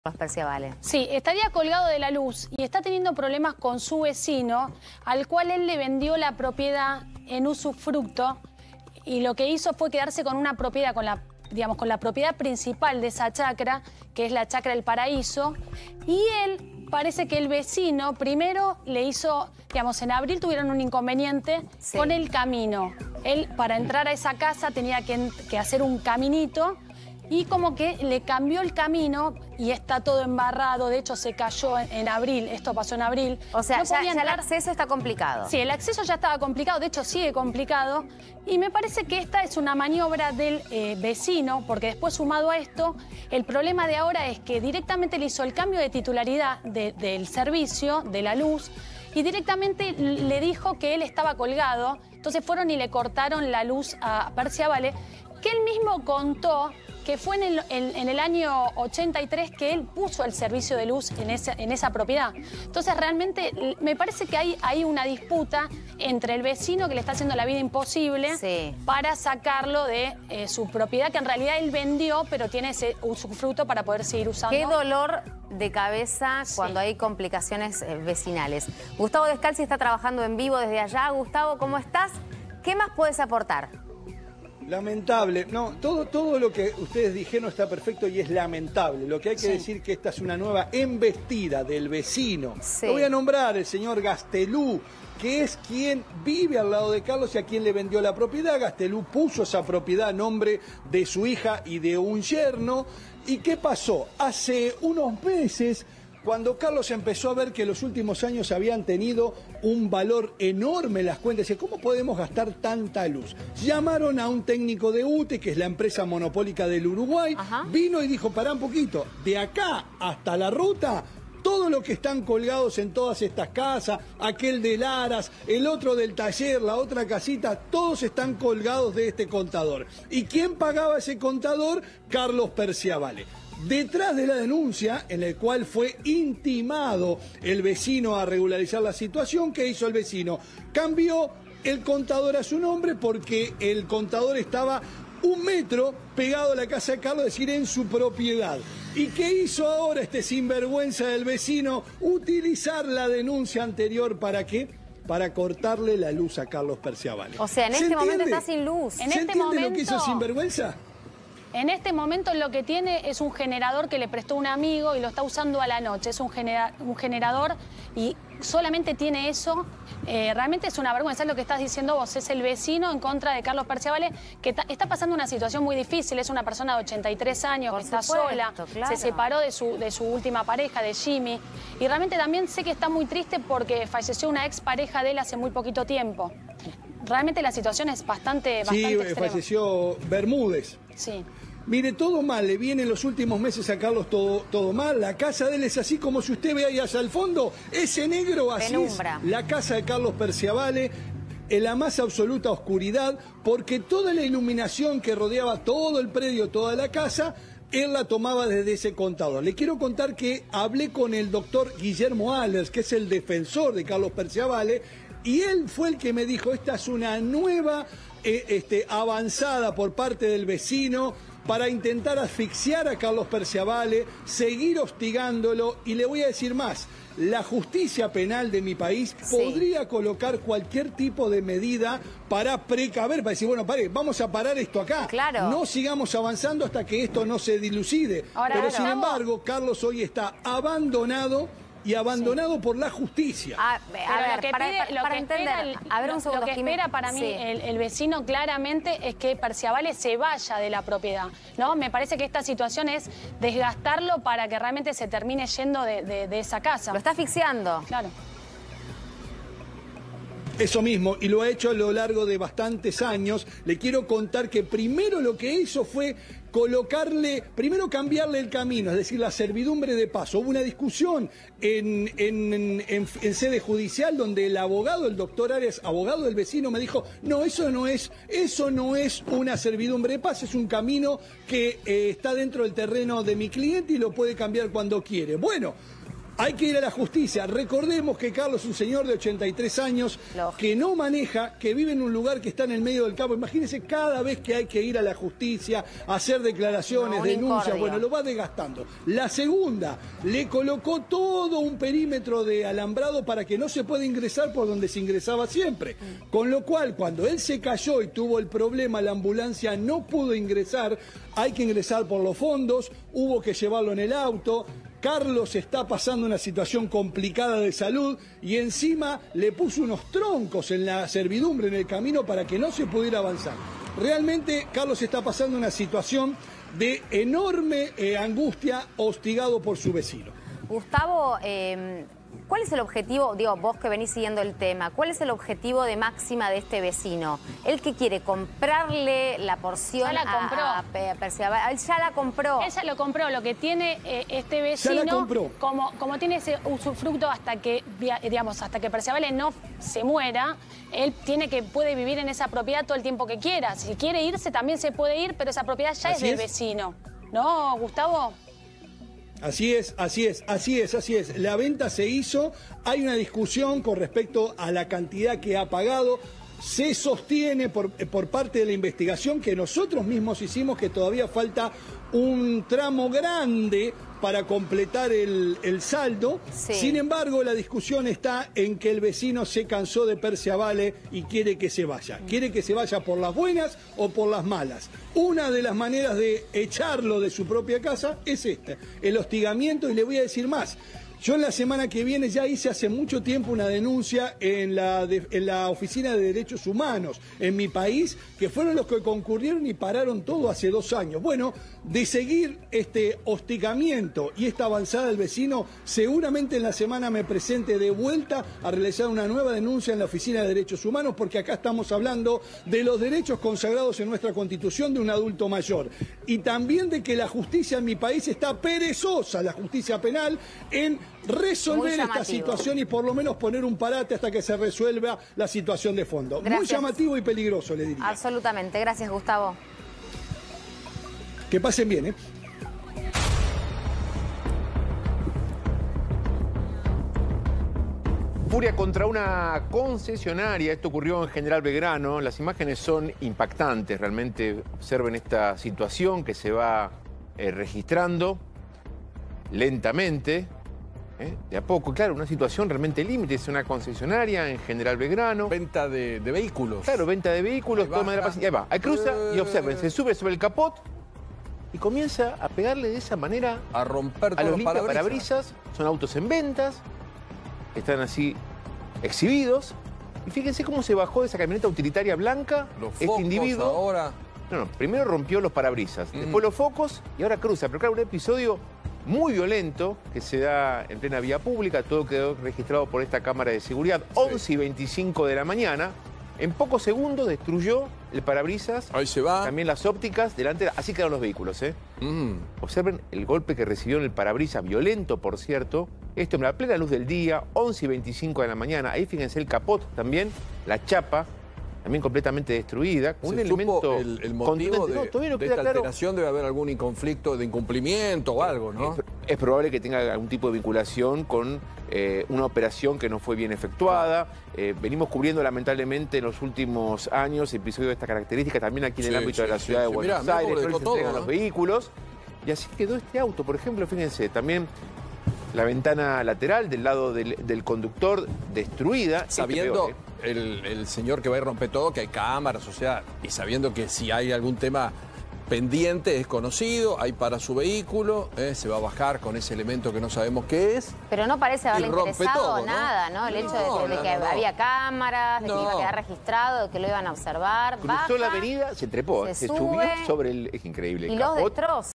📢 UN VECINO HOSTIGA A CARLOS PERCIAVALLE
👉 Carlos Perciavalle, famoso actor y humorista argentino , enfrenta una serie de problemas con un vecino que le compró una propiedad. La disputa comenzó con cambios en el camino de acceso a la casa de Perciavalle y ha escalado hasta el punto de que le han cortado la luz.